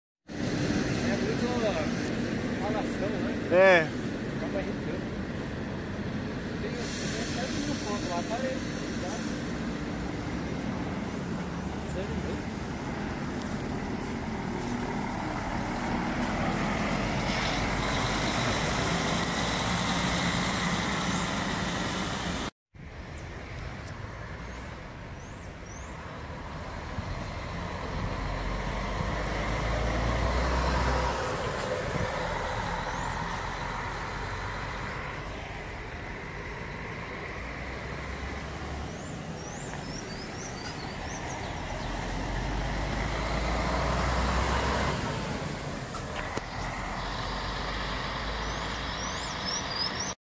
Rodoviária do Tietê SP 42
Primeiro carro
50150
Marcopolo Paradiso G7 1800DD
MBB O500RSDD blue tec 5
Viação águia branca
Segundo carro
2650
Marcopolo Paradiso New G7 1800DD
MBB O500RSD blue tec 5
Viação piracicabana
Terceiro carro
37363
Irizar I6 370
MBB O500RS blue tec 5
Airport bus service
Filmado na rodoviária do Tietê